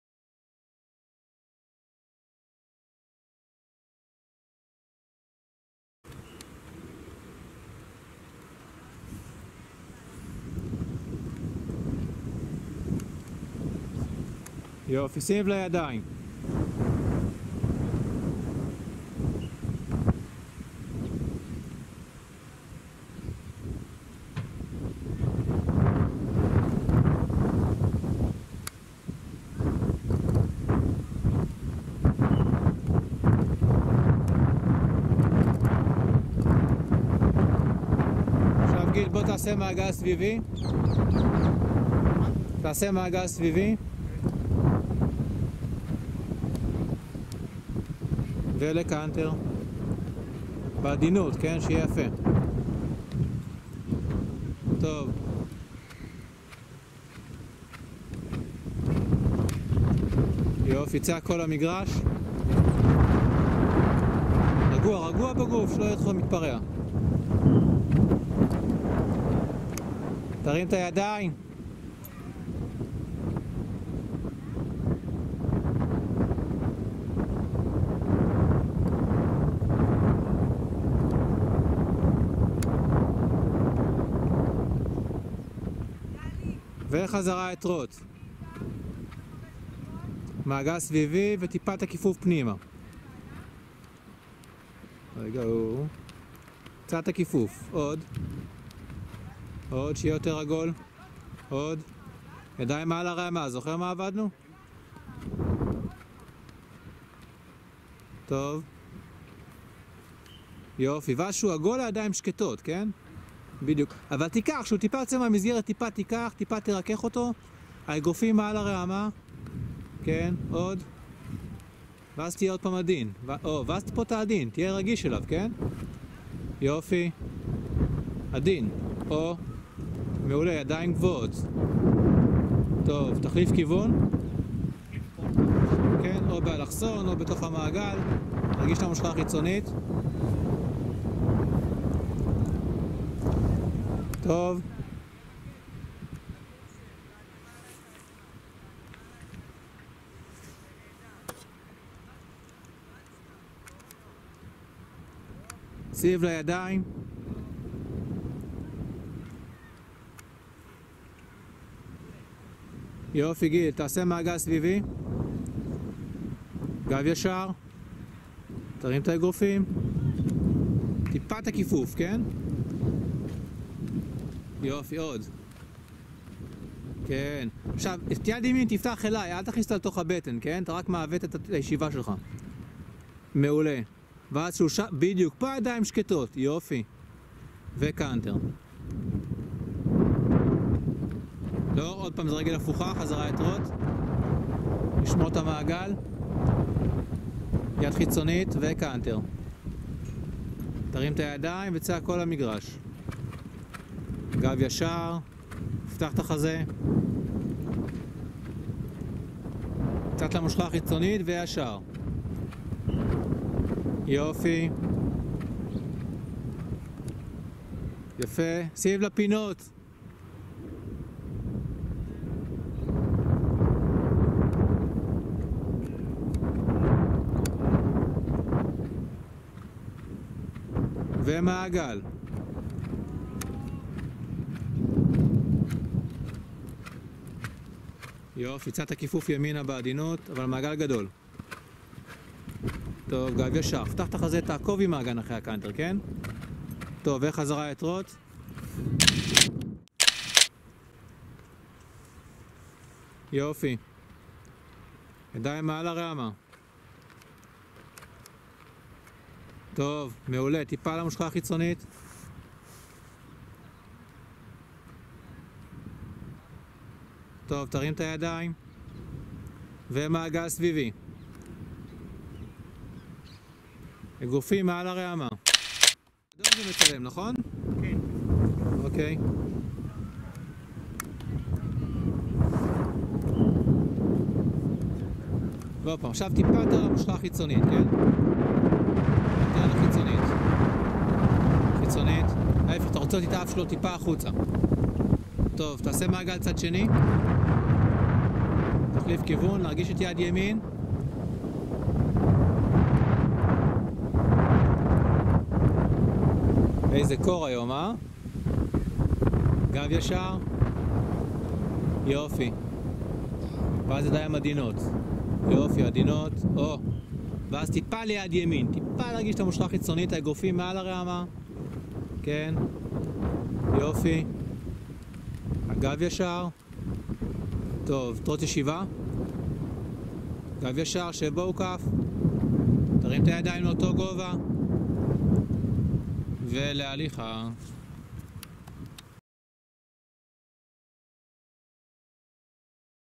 Whisky, school horse (jumper), 14 January 2018
Whisky, school horse (jumper), on 14 January 2018 at "Aviv" Stables in moshav Gan-Kha'yim (near Kfar-Saba), Israel.

Filmed by Omer (the instructor) with rider's camera; Edited by Gill (The rider).